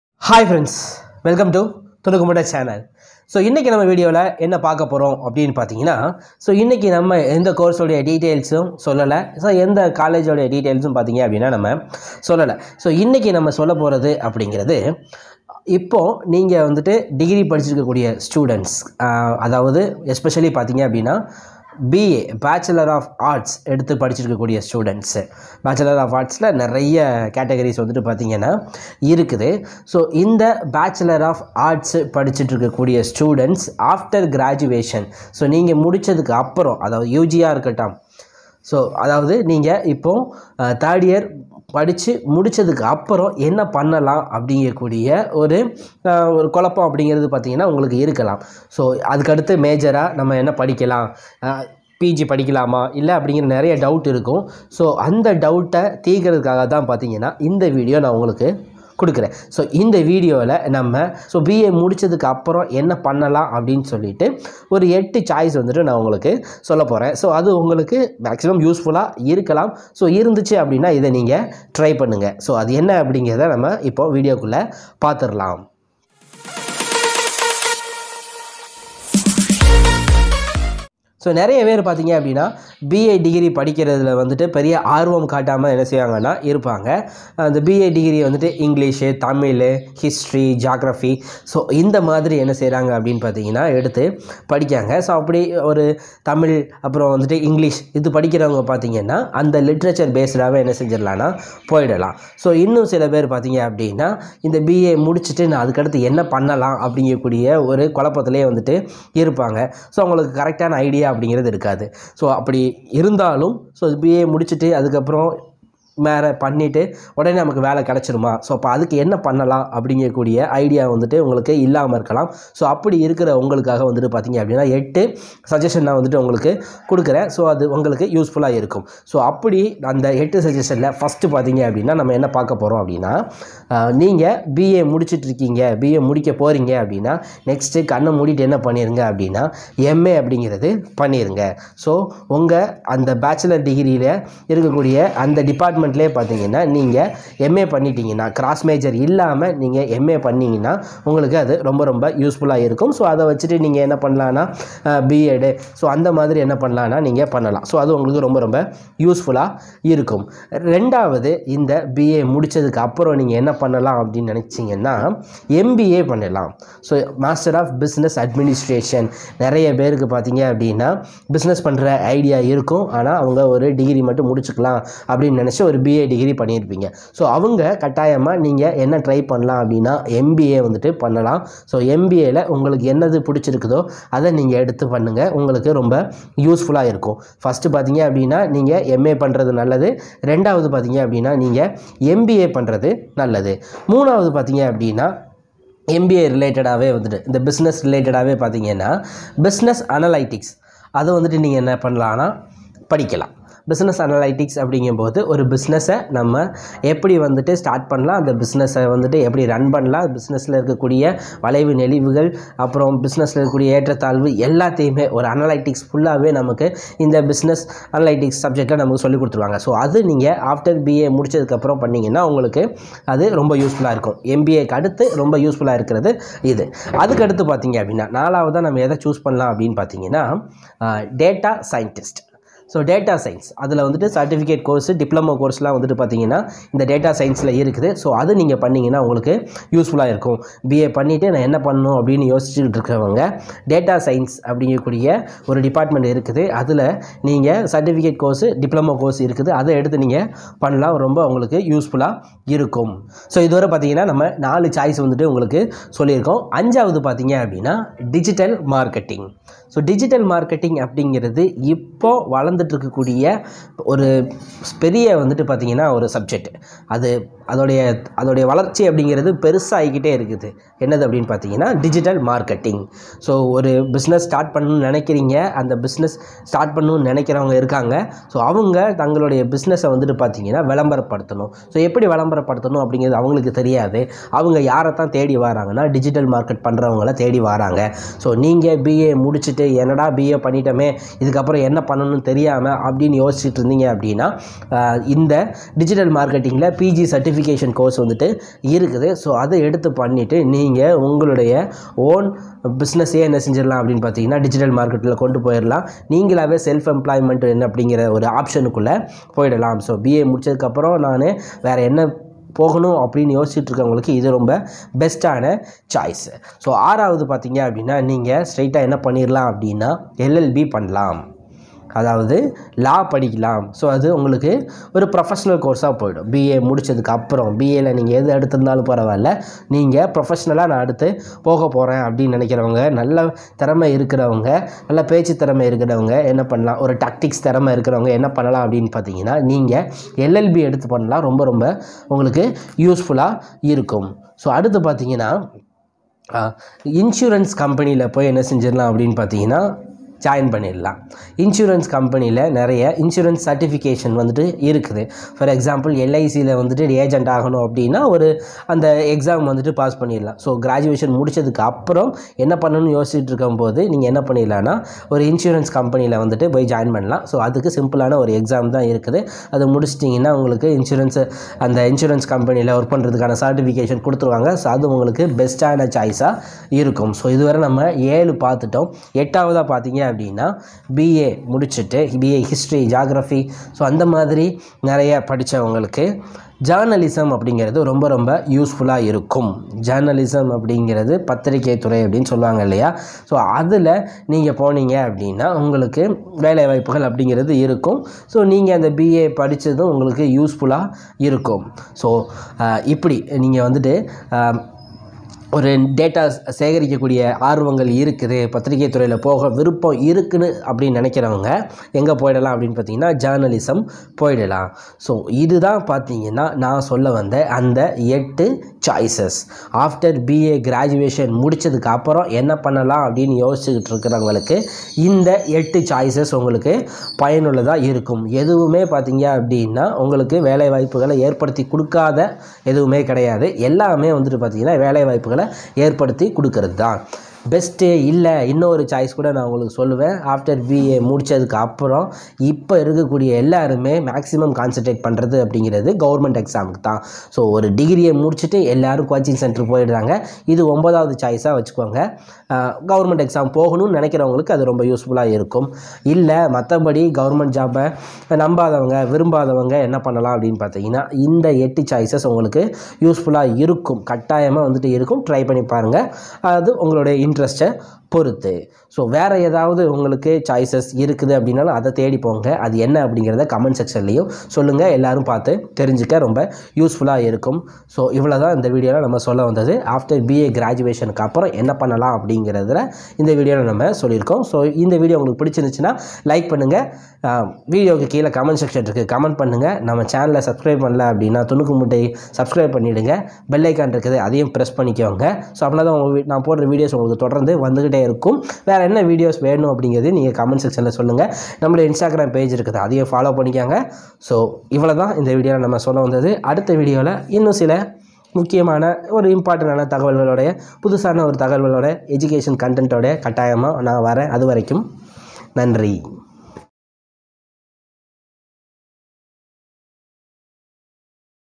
8 Options after |BA graduation |Best courses |Job oriented
This video gives you details about the best courses after BA graduation. This is very useful to the students who search for the best courses after the graduation.